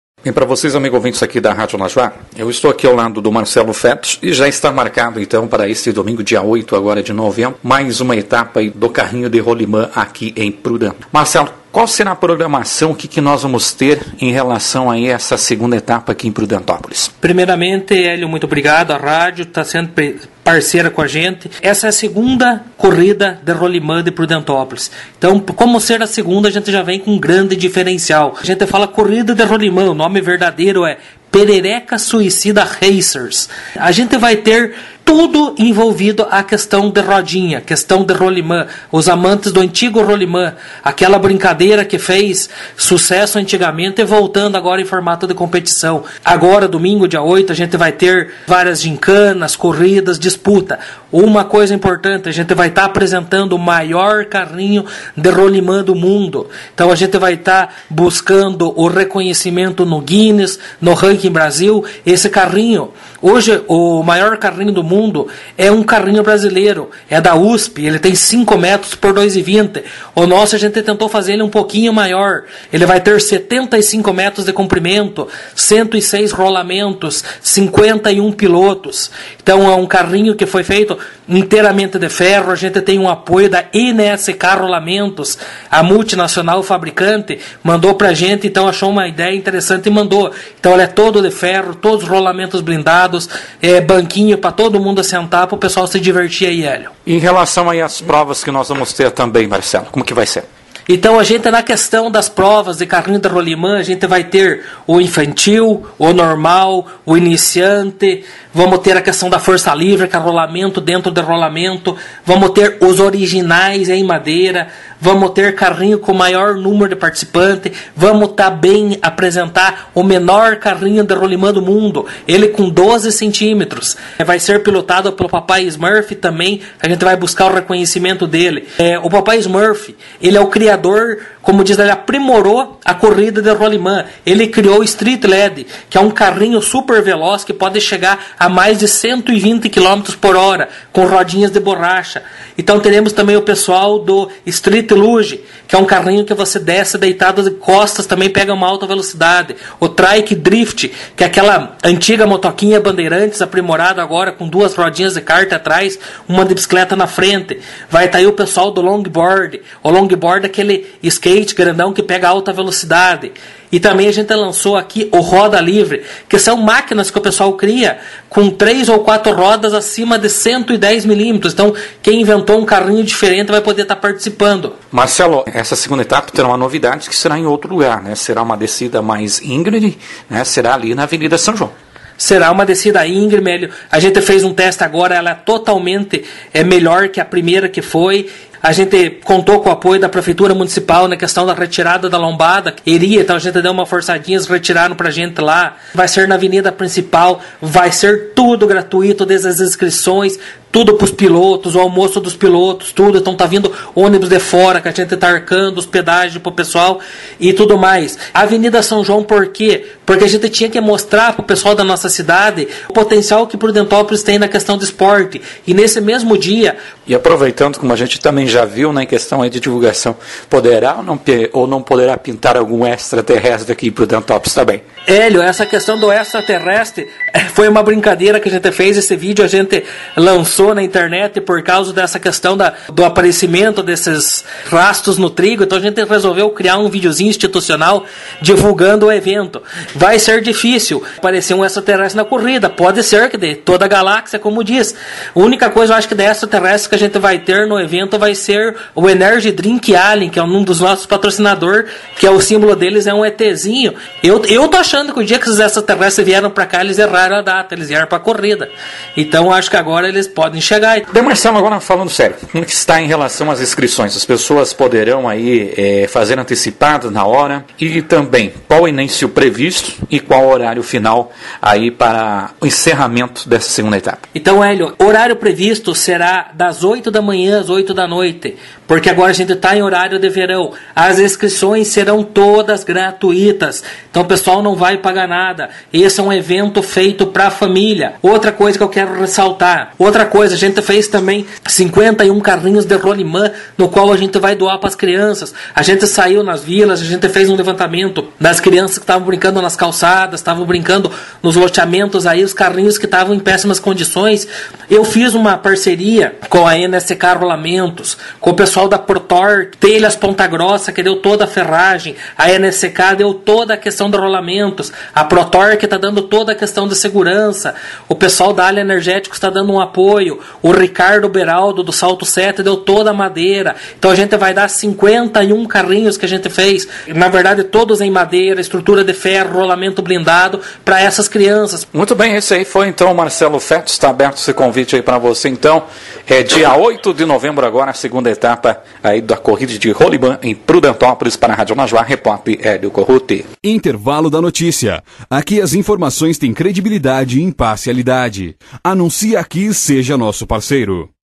Maior carrinho de rolimã do mundo será atração em Prudentópolis - Marcelo Fedus
(ESTE ÁUDIO É DE PROPRIEDADE DA RÁDIO NAJUÁ, ENTÃO É PROIBIDA A UTILIZAÇÃO SEM AUTORIZAÇÃO)

Com quase cinco toneladas, 75 metros de comprimento e 106 rolamentos, carrinho precisa ser pilotado por 51 pessoas neste domingo (08)